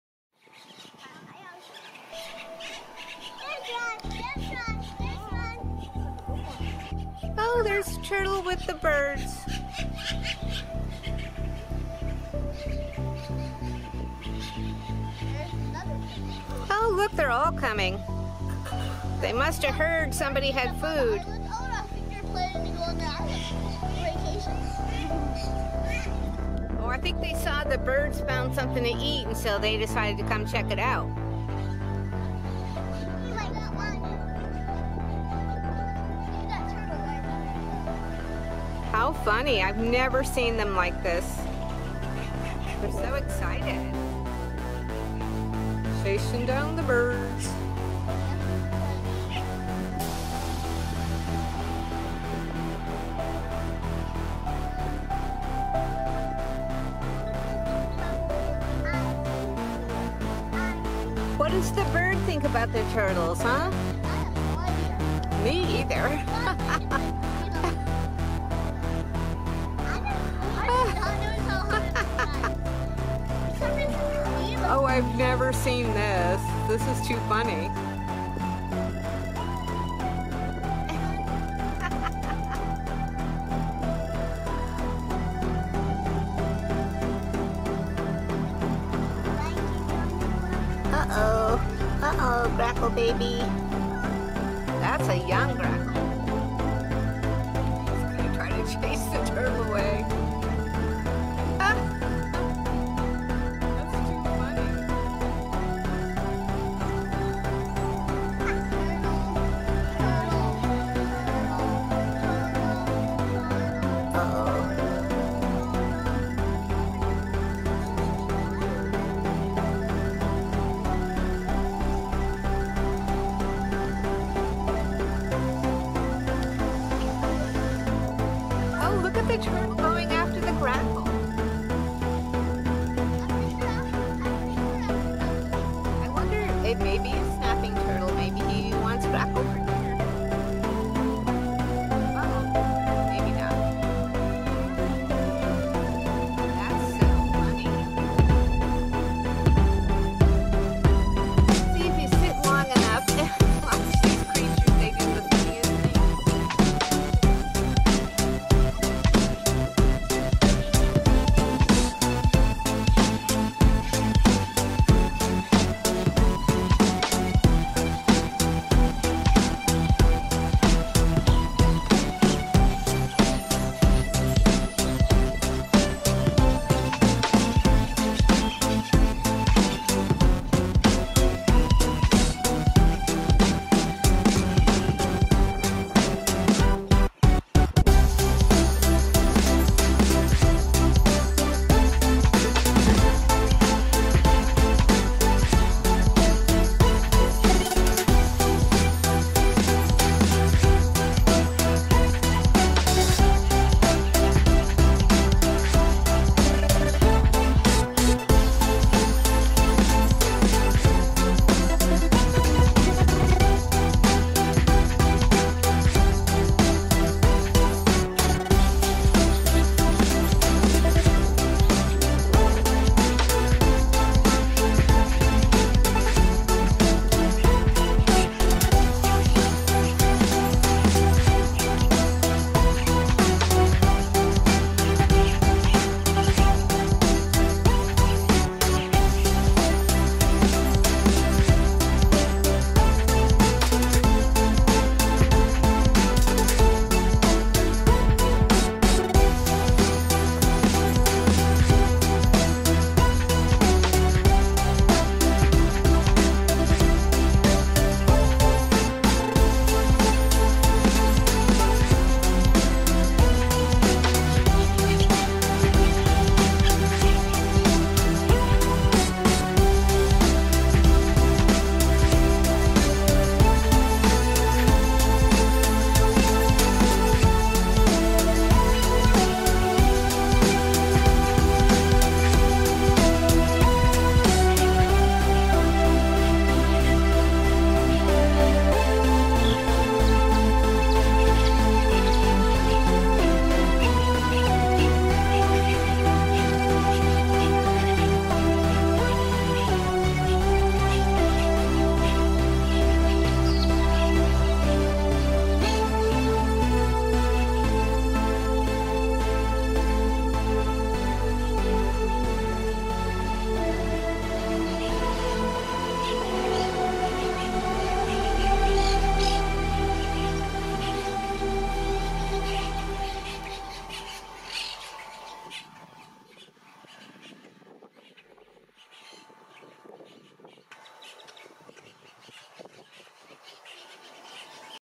Sometimes-Island Stand-Off, AustinWild!
I really didn't know what was going on between the turtles and grackle(s).  It seemed, at first, turtles were looking for food, which grackles seem to always have.
 No, that wasn't it, exactly.
Grackles are quite smart.  What they do is find leaves or sticks, dangle them in the water (or drop them for a second), to entice the fish.  Then grackles catch the fish.  Turtles are just checking if grackles might be dropping food, like french-fries (which grackles steal from eaters at the cafe), as the grackles prefer fish over fries, and turtles seem to not eat fish.  So, yes, turtle was looking for food, and grackles are a great food source.  Grackles are not scavengers- of the sort most would think.  They just use the human food to catch what they really want;  fish!